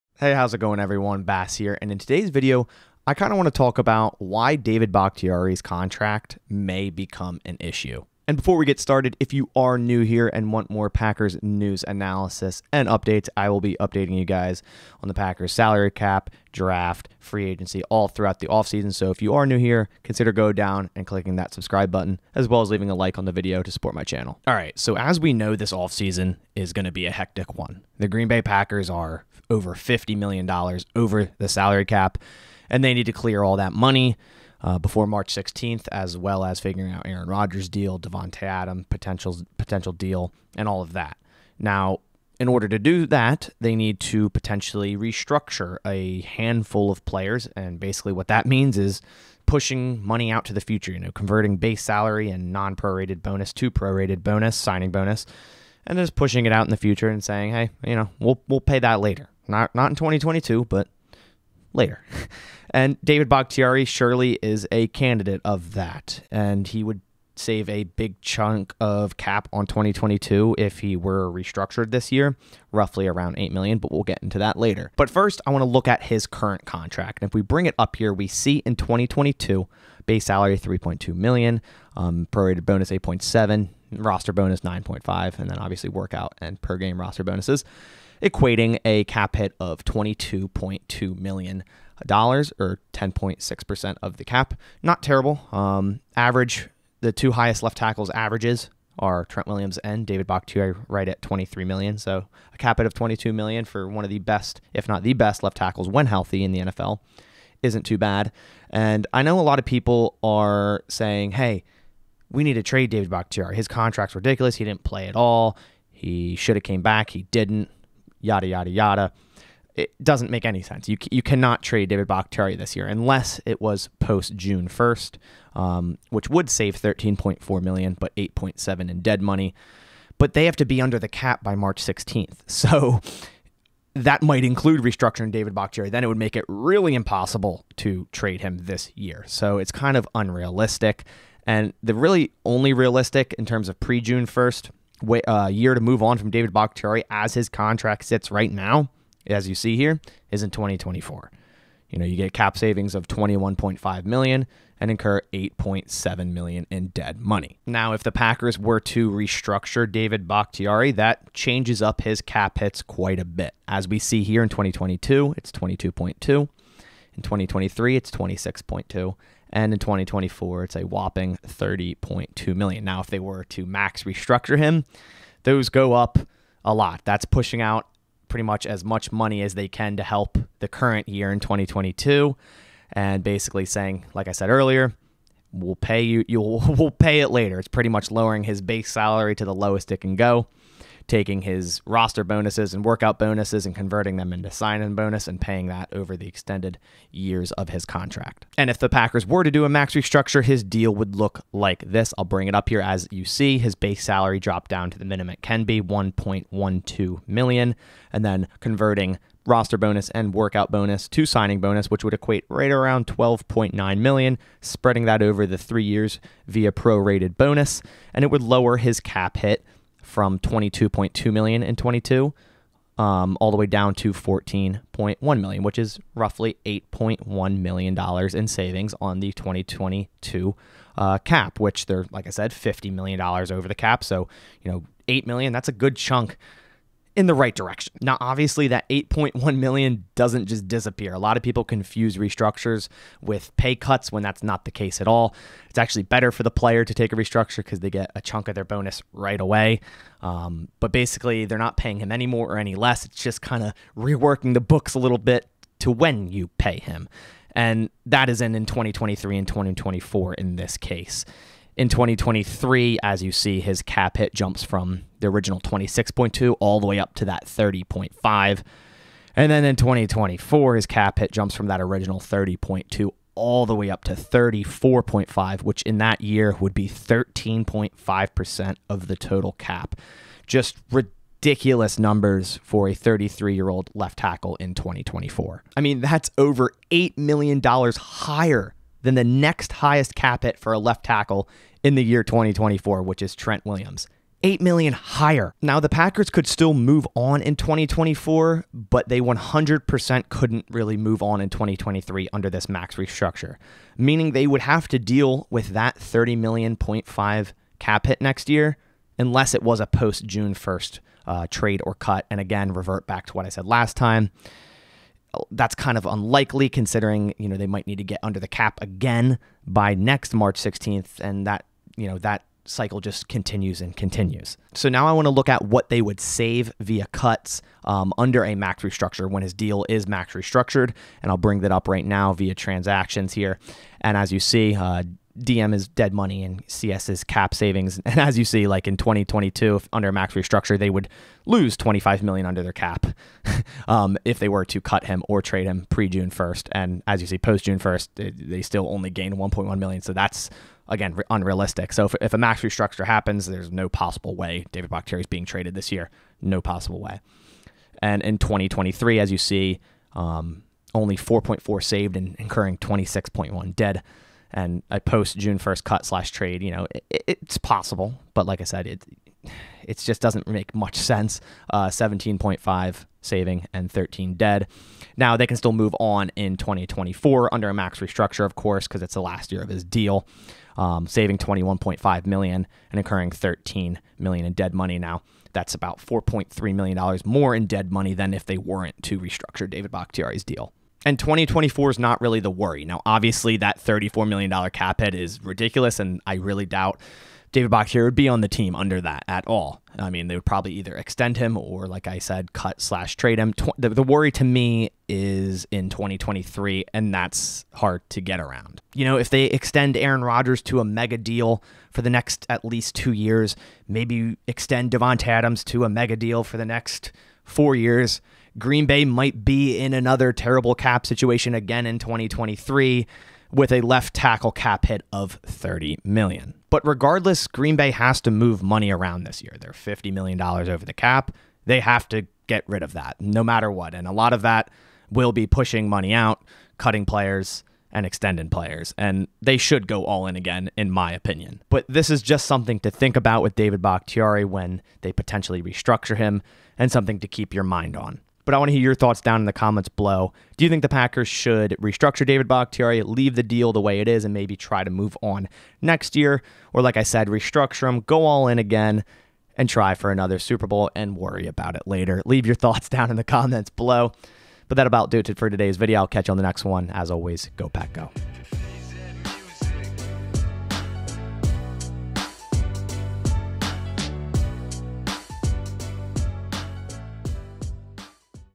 WHY David Bakhtiari's Contract May Become an Issue
Want to talk a bit today about the future of David Bakhtiari's contract for Green Bay